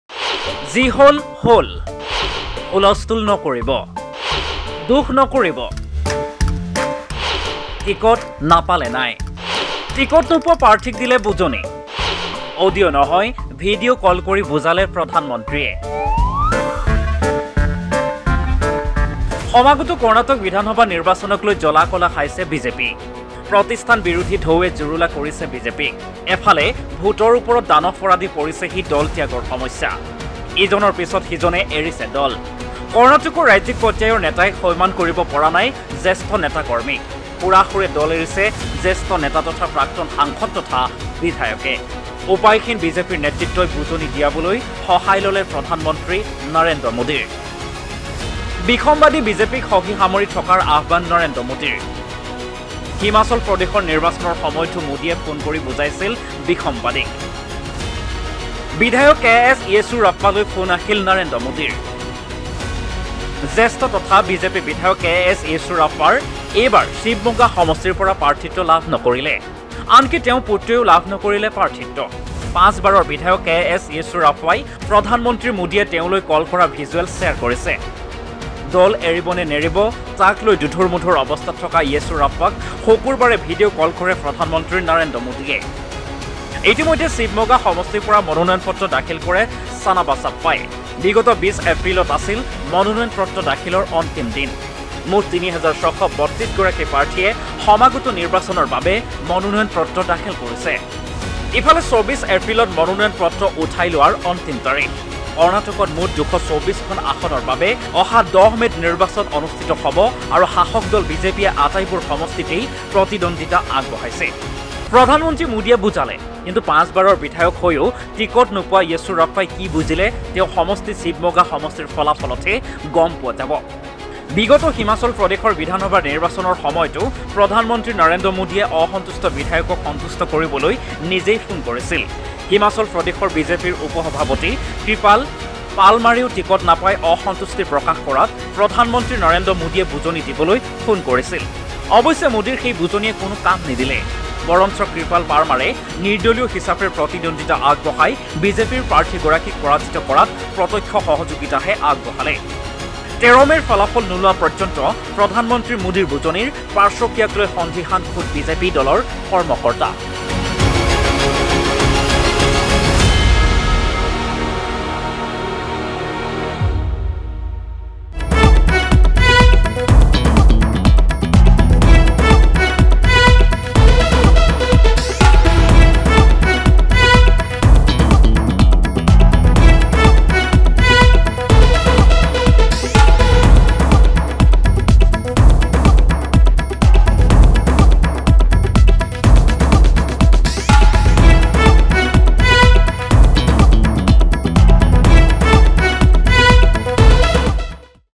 টিকেট নোপোৱা প্ৰাৰ্থীক দিলে বুজনি, অডিঅ’ নহয়,ভিডিঅ’ কল কৰি বুজালে প্ৰধানমন্ত্ৰীয়ে
'যি হ’ল ,হ’ল ,হুলস্থূল নকৰিব ! দুখ নকৰিব ! টিকেট নাপালে নাই '